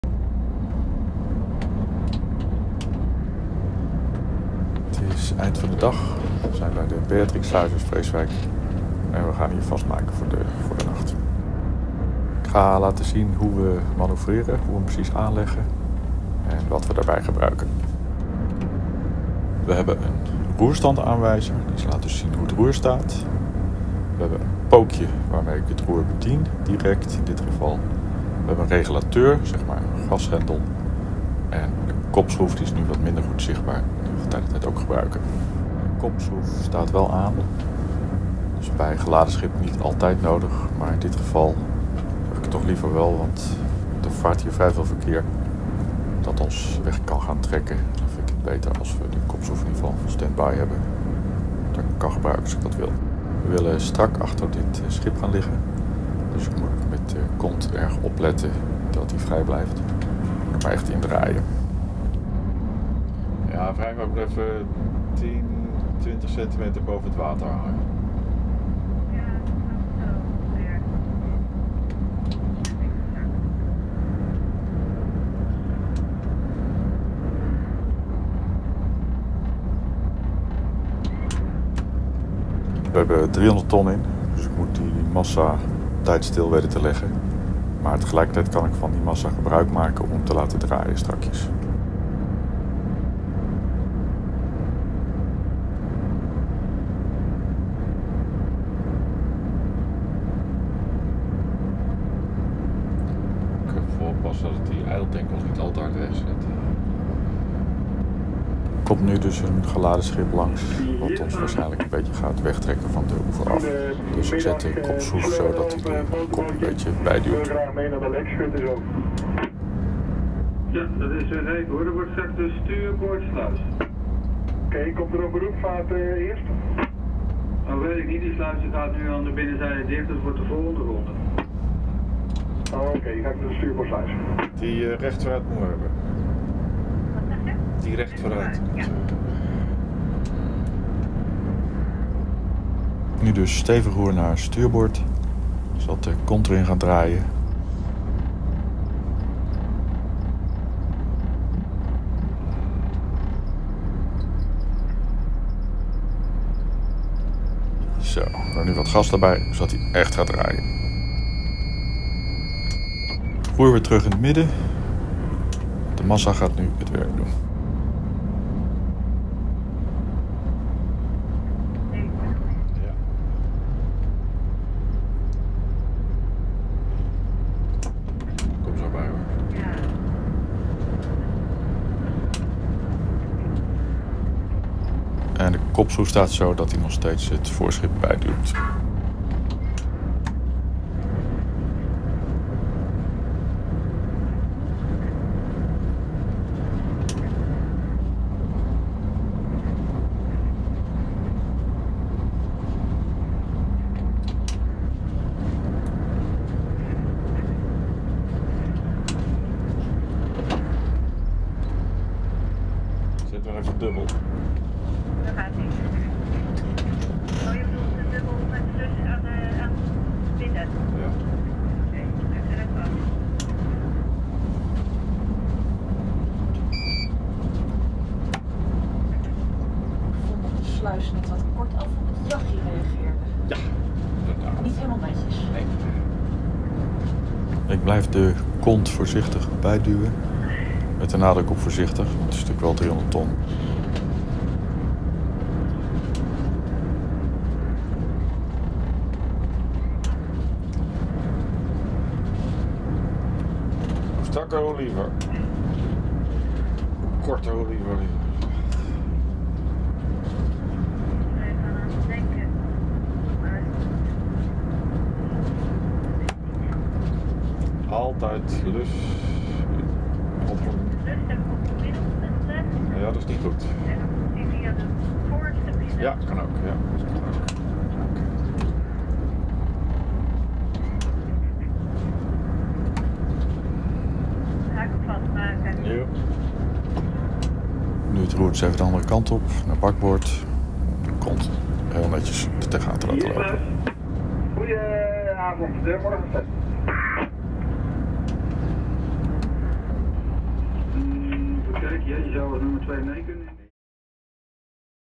Dinsdag 27 April 2021 (2/2) - Aanleggen voor de nacht bij de Beatrix sluizen
IJmuiden - Evry, Coils

Aanleggen met geladen schip, met de roerstanden, regulateur, kopschroef, schroefwerking en wat er nog meer bij komt kijken

Het maken van deze blogs en onze filmpjes op YouTube gaat niet vanzelf.